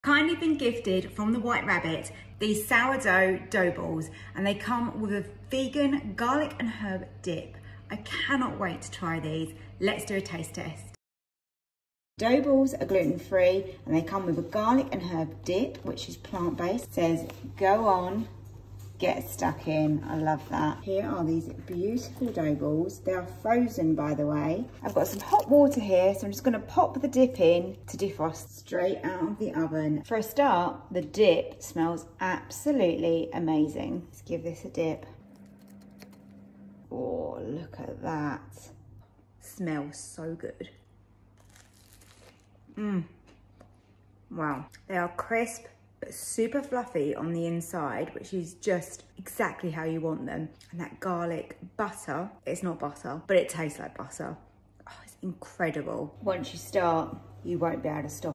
WHITE RABBIT Sourdough DOUGH BALLS with GARLIC & HERB DIP | Gluten Free & Plant Based VEGAN *Gifted
The WHITE RABBIT have kindly gifted me their Gluten Free Dough Balls with Vegan Garlic & Herb Dip
Bite Size perfection , fluffy in the middle & the dip is mouthwatering

Lets Taste Test...